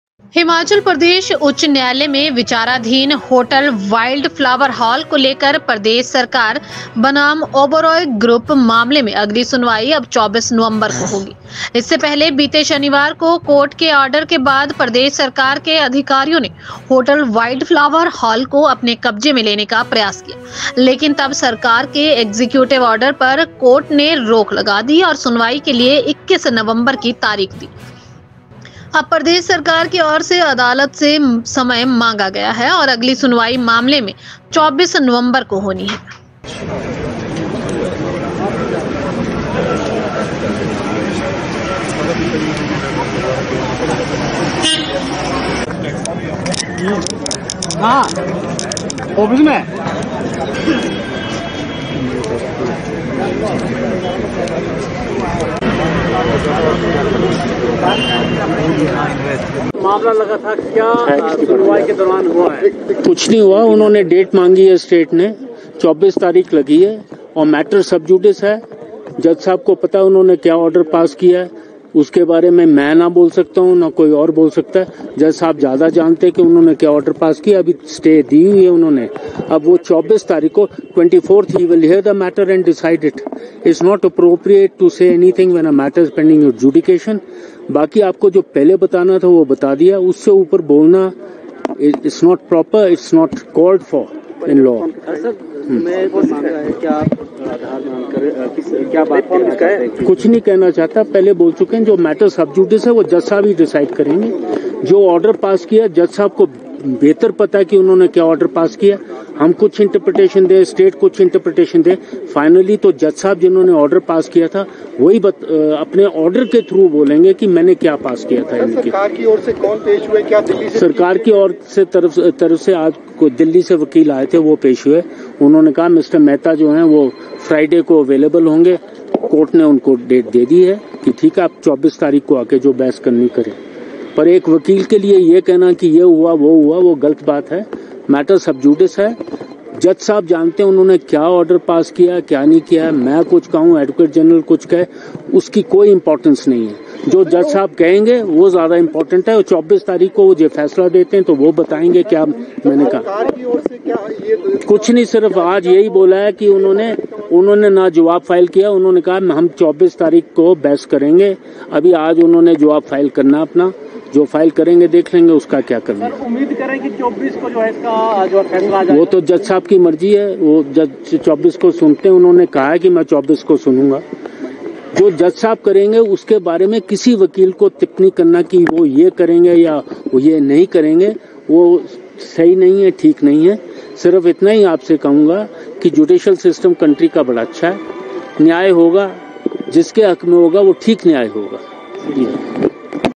High Court | Wildflower Hall | Chharabra
शिमला। हिमाचल प्रदेश उच्च न्यायालय में विचाराधीन होटल वाइल्ड फ्लावर हॉल को लेकर प्रदेश सरकार बनाम ओबरॉय ग्रुप मामले में अगली सुनवाई अब 24 नवंबर को होगी। इससे पहले बीते शनिवार को कोर्ट के ऑर्डर के बाद प्रदेश सरकार के अधिकारियों ने होटल वाइल्ड फ्लावर हॉल को अपने कब्जे में लेने का प्रयास किया लेकिन तब सरकार के एग्जीक्यूटिव ऑर्डर पर कोर्ट ने रोक लगा दी और सुनवाई के लिए 21 नवंबर की तारीख दी। अब प्रदेश सरकार की ओर अदालत से समय मांगा गया है और अगली सुनवाई मामले में 24 नवंबर को होनी है।